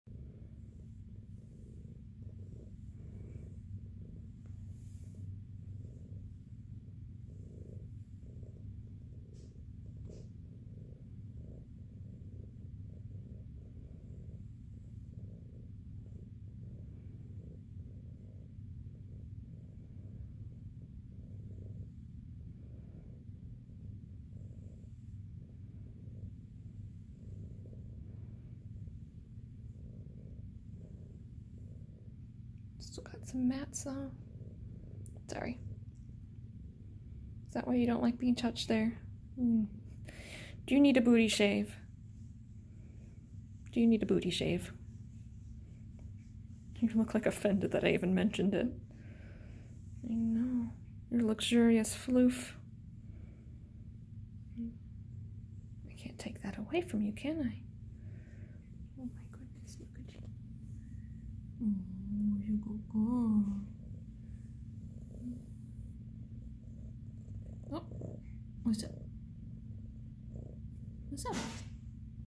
Shelter Cat Jiji Horribly Offended at the Simple Mention of Trimming Her Luxurious Floof! [REAL]
"Hi, I’m Jiji. I originally came to SBACC as a two-month-old kitten and was adopted about two months later.  My adopters were a young couple with one other cat.  The other cat and I were OK together but never became buddies.  Then a new baby arrived and it just didn’t go well.  My Mom and Dad tried hard to make it work but finally decided to bring me back to SBACC.  I was really scared and very unhappy and had a hard time trusting new people but I’m finally starting to adjust.  I’m a big fluffy girl with soft black fur and a white triangle on my chest.  I’m sassy and smart and can be lovable once I get to know you.  I enjoy being petted and brushed if you do it very gently.  My favorites are neck, cheek, and chest scratches. If I really like ya, I may even allow belly rubs!  I like when people talk to me softly or even when they sing.  Sometimes I will talk back.  I’m a big girl but I have a tiny little meow.  I enjoy the laser and other toys.  I think I would do best as an only cat in a quiet home without any dogs or little children."